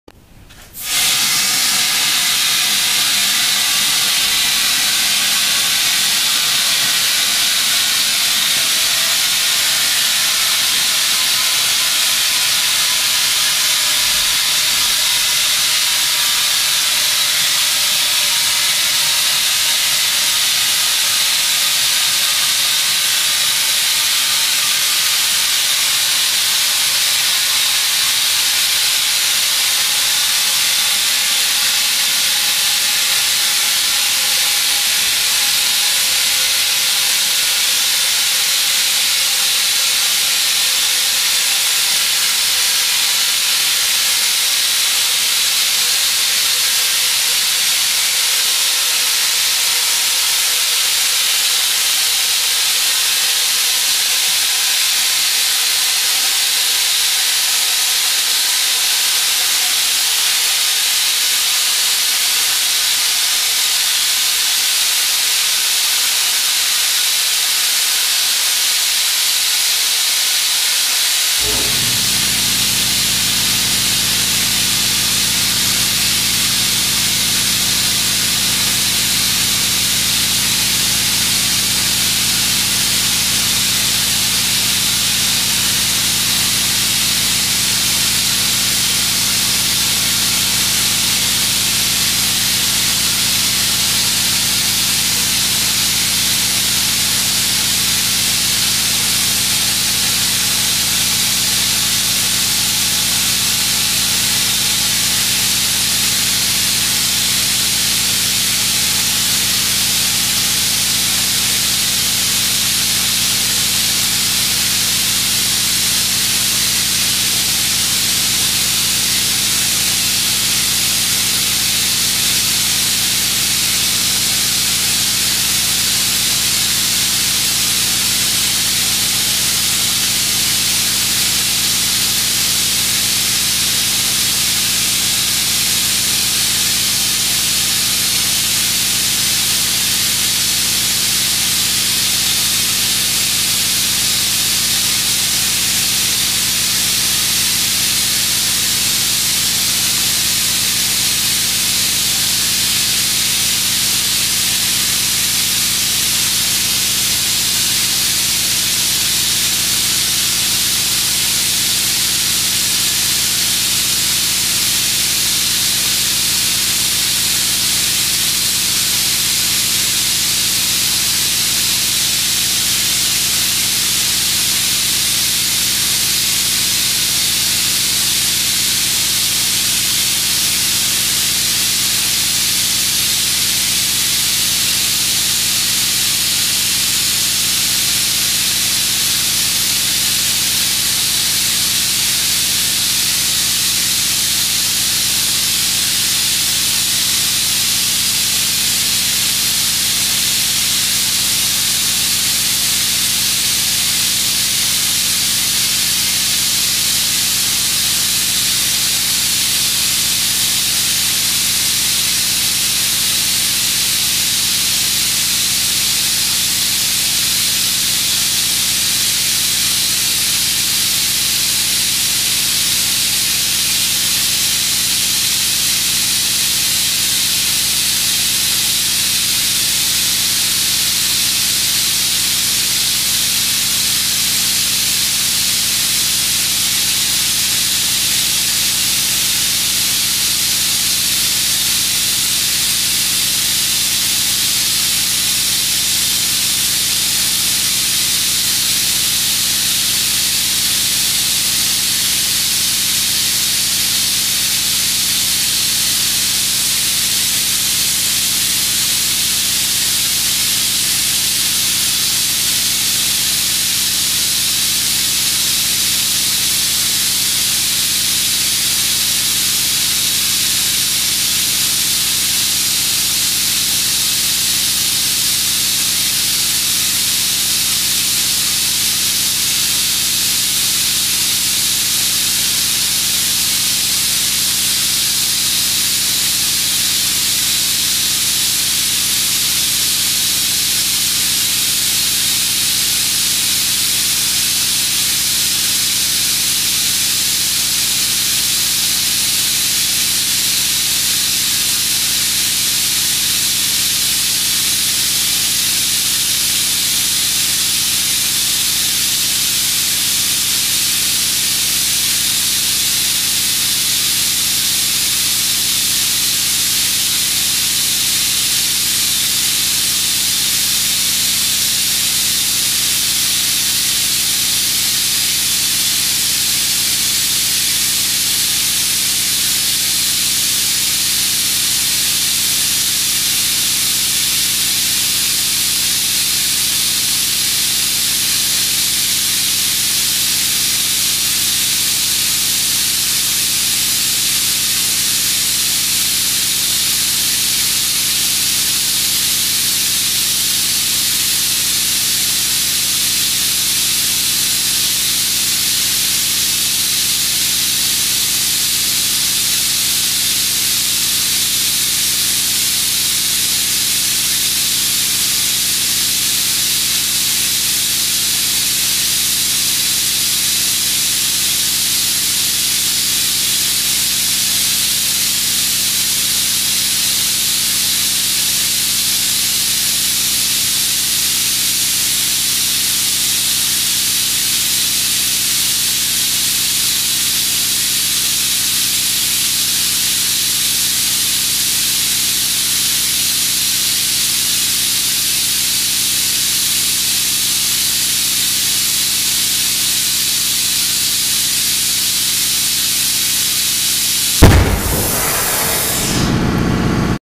Inner Tube Burst 276
13.6R14.9R28/30 - This one was over 9 feet in diameter.